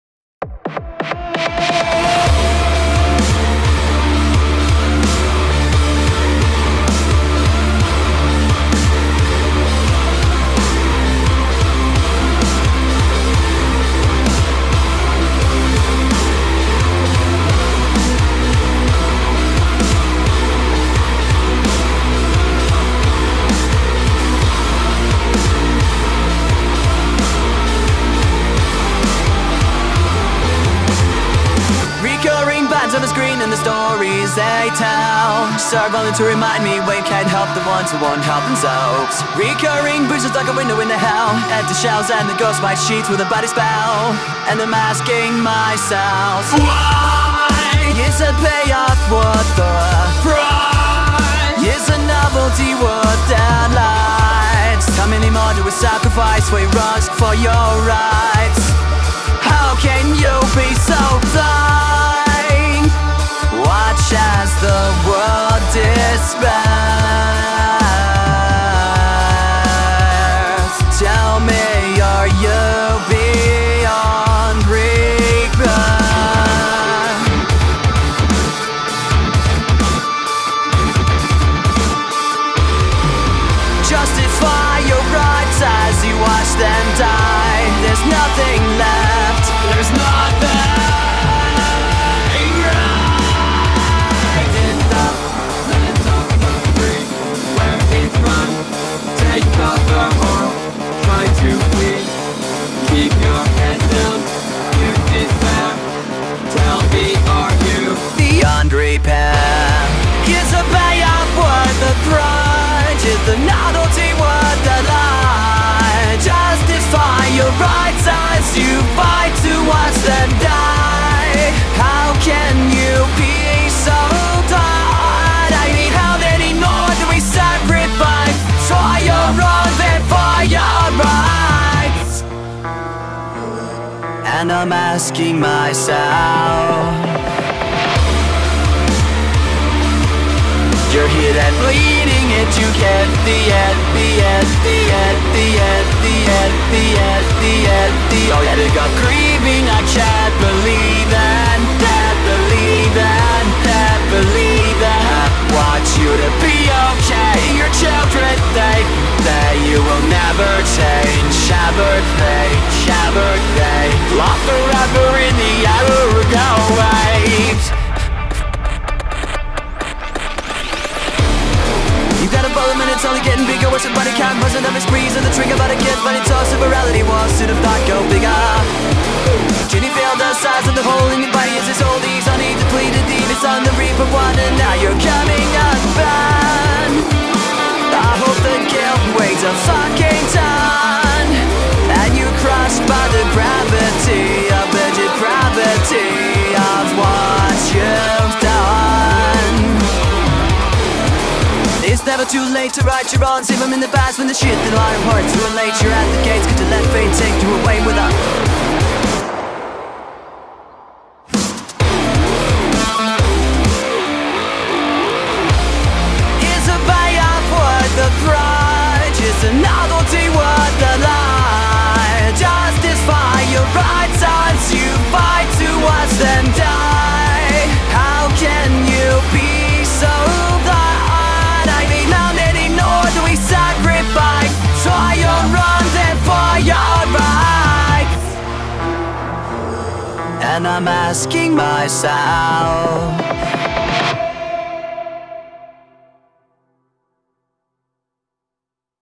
Chester Bennington AI - Voice Model Sample
Test song for  Chester Bennington AI Model Trained By Me

I did add distortion to some parts of the vocals.

© Licence ▼

Check out the artist's pages:
➥ SETMEONFIRE